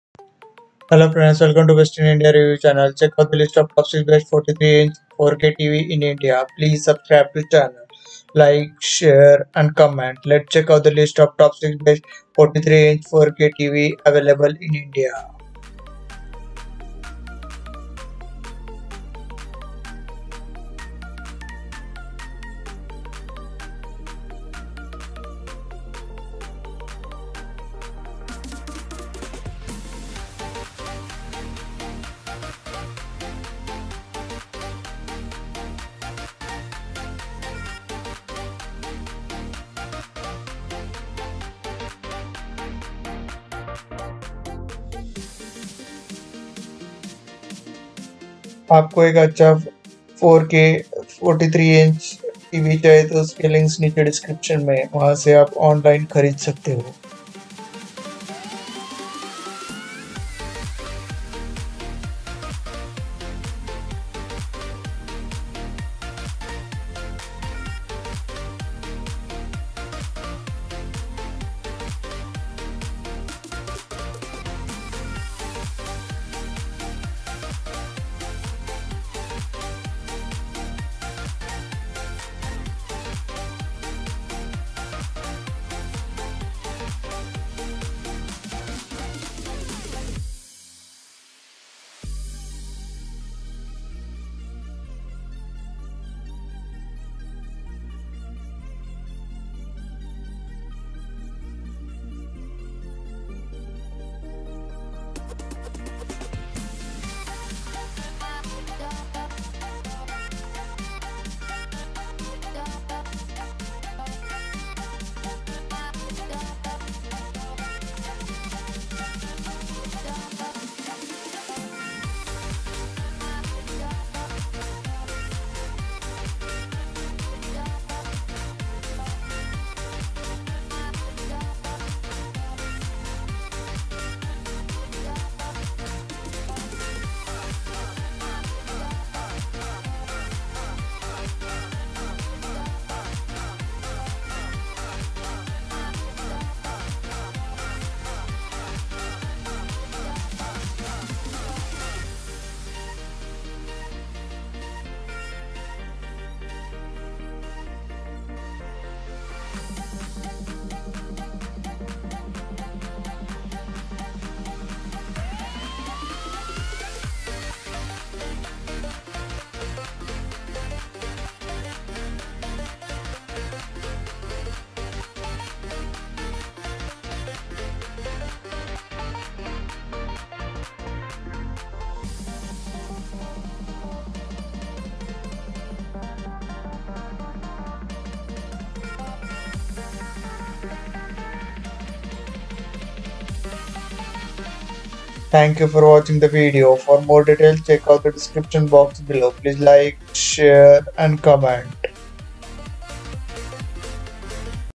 Top 6 Best 43 Inch 4k TV in India | BEST 43 INCH SMART TV IN INDIA - 43 इंच एलईडी 4K टीवी
Top 6 Best 43 Inch 4k TV in India - 43 इंच एलईडी 4K टीवी

List of Top 6 Best 43 Inch 4k TV in India

1. Sony Bravia Smart Android LED TV (KD-43X74)

2. LG 4K Ultra HD Smart LED TV (43UP7500PTZ)

3. Hisense 4K Ultra HD Smart Android LED TV

4. Acer 4K Ultra HD Android Smart LED TV

5. Kodak 4K Ultra HD Smart LED TV (43CA2022)

6. iFFALCON 4K Ultra HD Smart LED TV (43U61)

Other Televisions:

Searching for a Best 43 Inch 4k TV Online then here is the list of Top 6 Best 43 Inch 4k TV.
Which is the best 43 inch 4K TV?